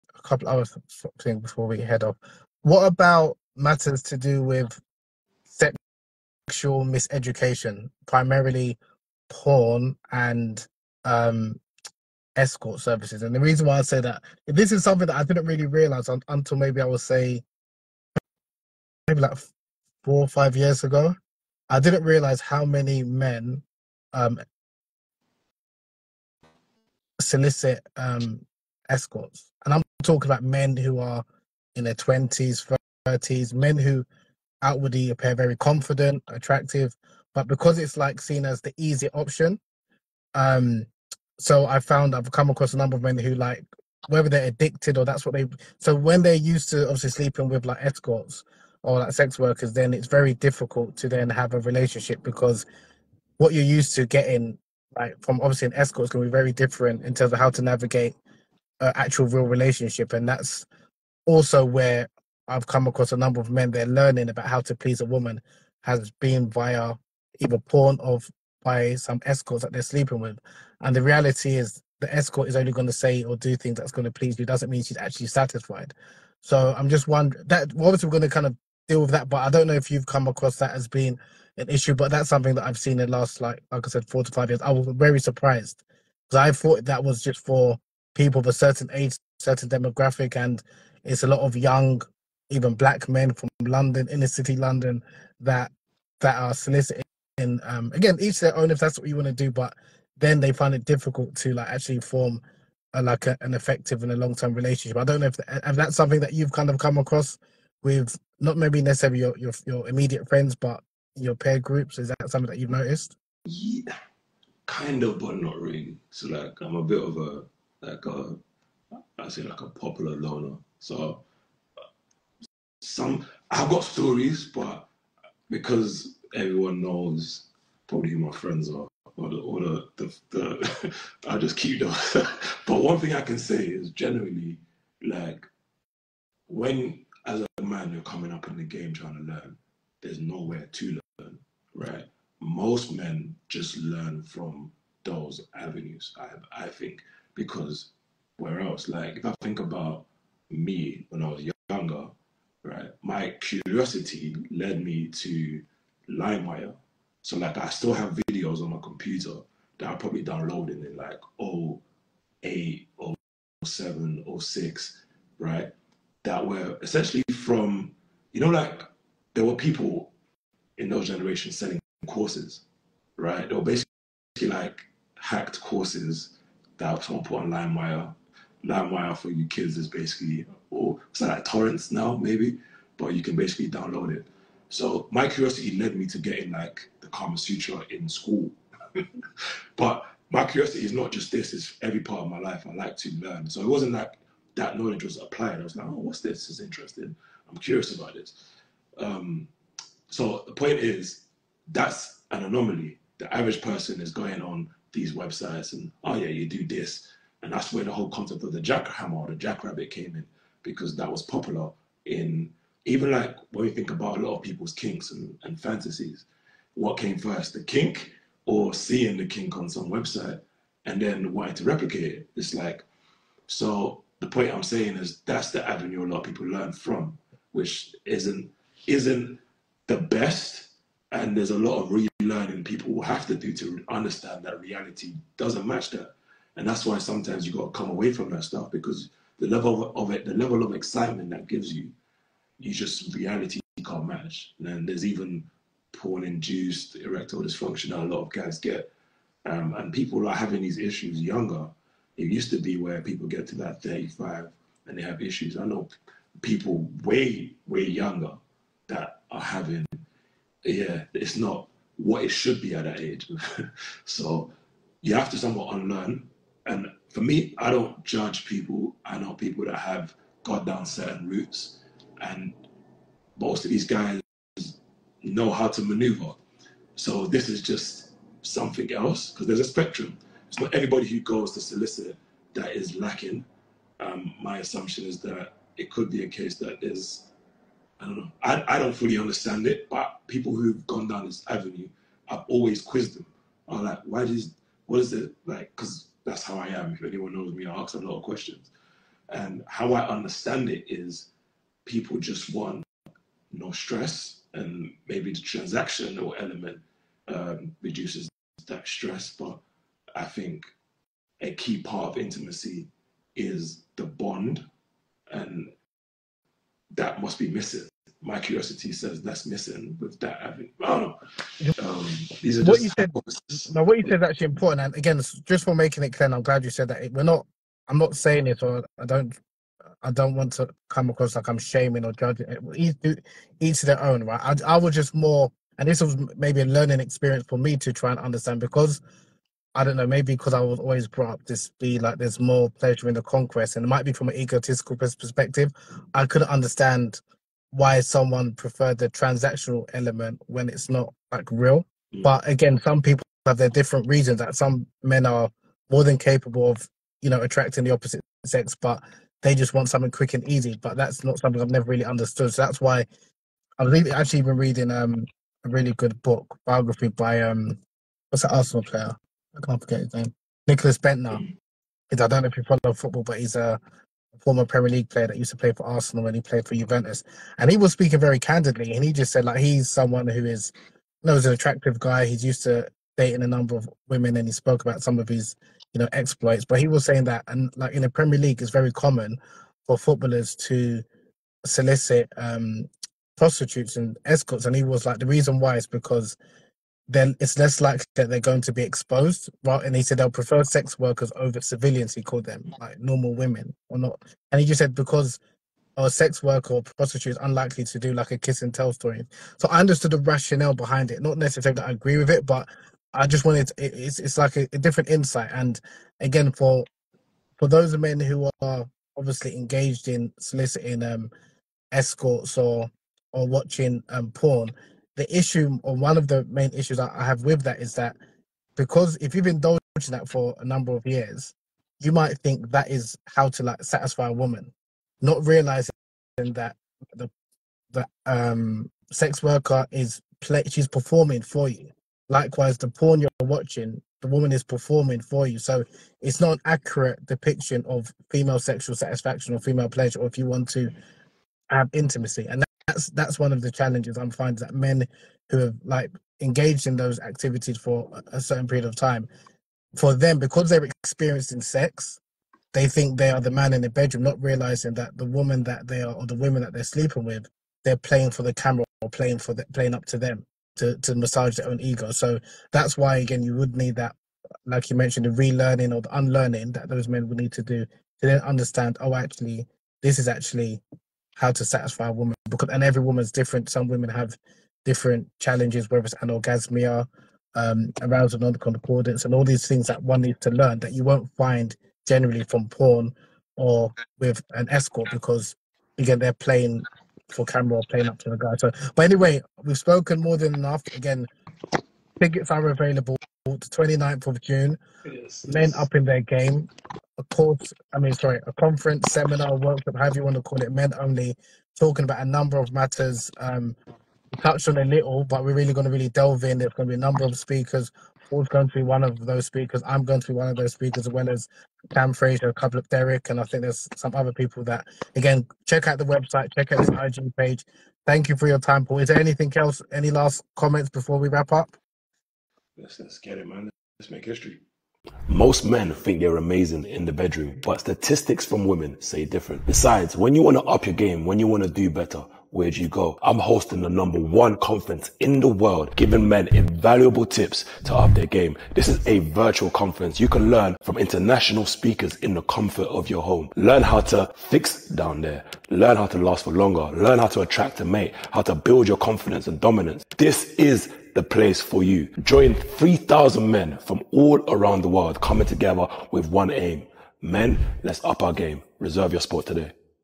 Why Footballers Pay For Sex
Discussion between Habeeb Akande and Paul Otote @DunguBook about porn addiction and why footballers prefer escorts and young men pay for sex.

According to former premier league footballers, Nicklas Bendtner and Louis Saha, Premier League footballers prefer escorts than sleeping with non-sex workers. Former Arsenal player, Bendtner claimed that footballers prefer hiring sex workers to one-night stands with 'civilians' as there's no risk of later getting blackmailed." Former Manchester United player, Saha, told a French newspaper while promoting his new autobiography:

"Let's be honest: women are the greatest temptation. For a young player, it's an achilles heel. They can quickly be taken in, seduced by the girl who will cash in with a kiss-and-tell to the newspapers. So it's hard to know who to trust and you become paranoid where women are concerned. Some players therefore prefer to use escorts."
Saha revealed in his book that some footballers are sex addicts, and even use hookers. The former Tottenham Hotspur striker said that kiss-and-tell girls make players "paranoid about women". "As a result, some among them resort to using escort girls instead."

Whilst soliciting escorts may be widespread amongst footballers and young men, it can lead to a detrimental effect on men and their relationships, including sexual dysfunction and sexual miseducation.

Paul Otote is a herbalist, author and founder of the Men Upping Their Game virtual conference taking place on 29 June 2024